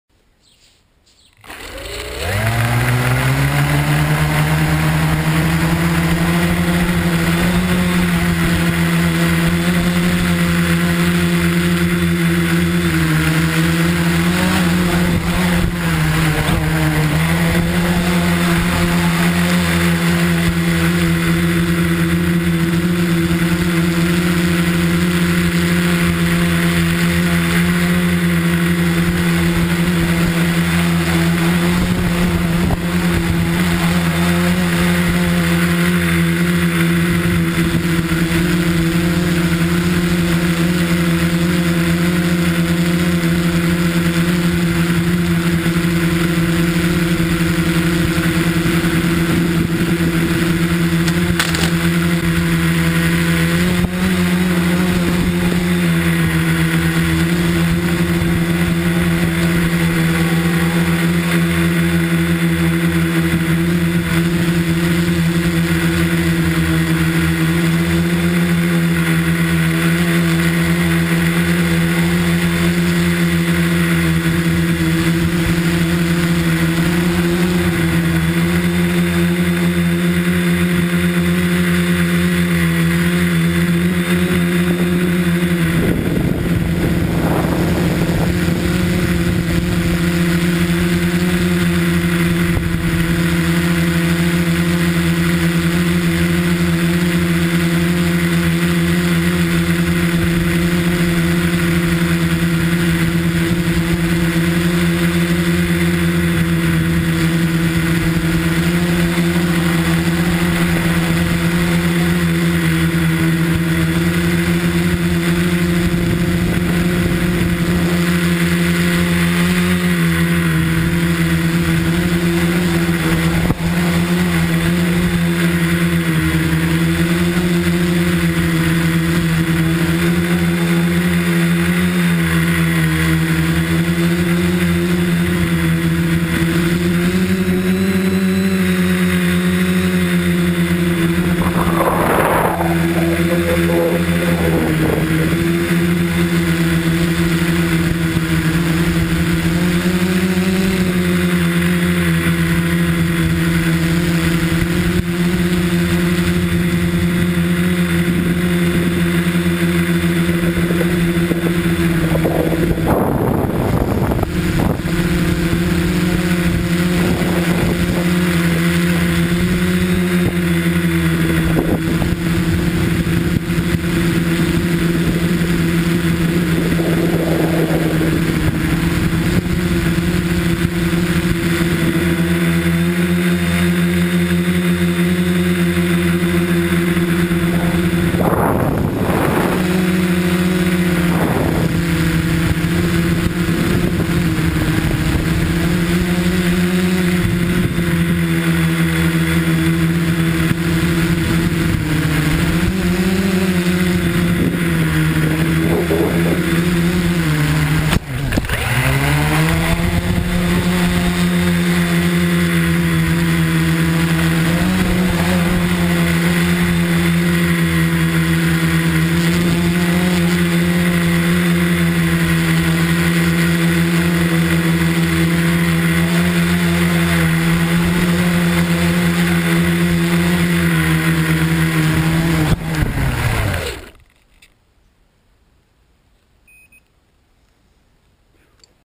FPV Tricopter - Low and Below - HD
Working on flying very low below trees with the tricopter.
Video filmed with GoPro Hero HD